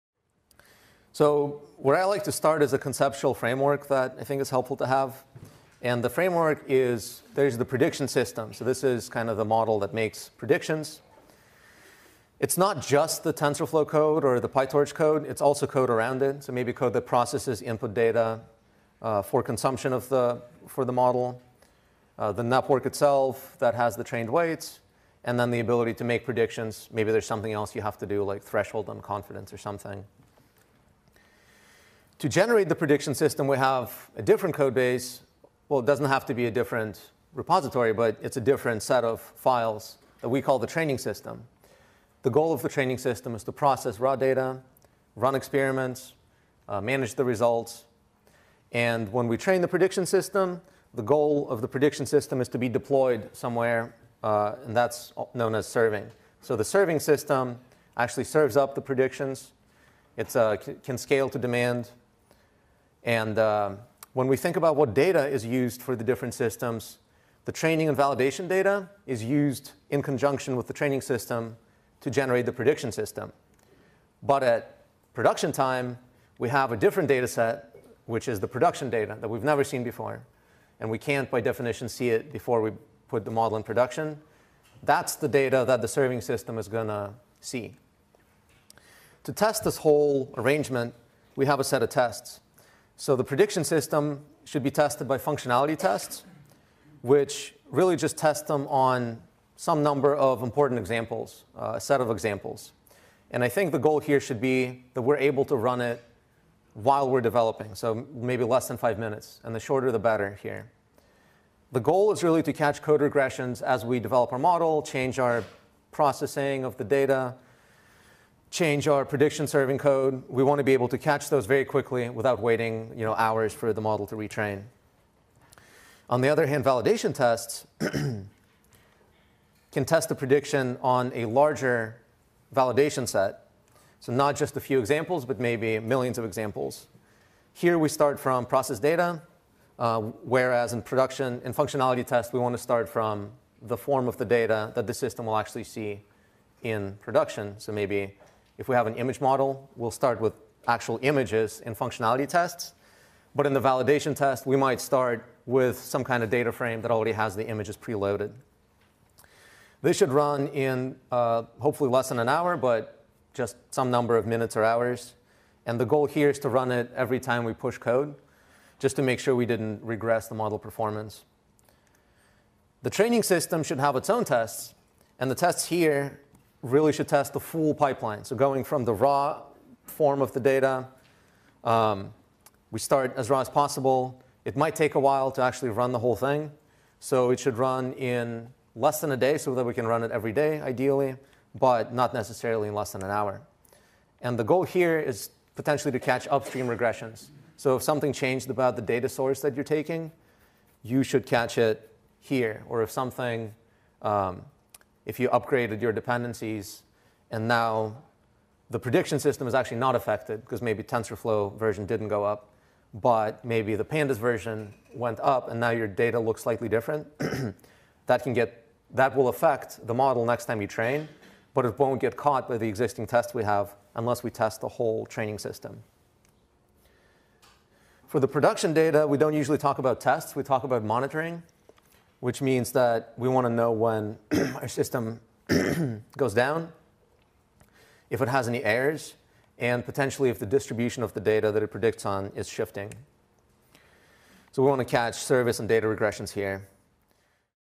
Project Structure (1) - Testing & Deployment - Full Stack Deep Learning
What are the different components of a machine learning system?

Summary
- The prediction system involves code to process input data, to construct networks with trained weights, and to make predictions.
- The training system processes raw data, runs experiments, and manages results.
- The goal of any prediction system is to be deployed into the serving system. Its purpose is to serve predictions and to scale to demand.
- Training and validation data are used in conjunction with the training system to generate the prediction system.
- At production time, we have production data that has not been seen before and can only be served by the serving system.
- The prediction system should be tested by functionality to catch code regressions and by validation to catch model regressions.
- The training system should have its tests to catch upstream regressions (change in data sources, upgrade of dependencies)
- For production data, we need monitoring that raises alert to downtime, errors, distribution shifts, etc. and catches service and data regressions.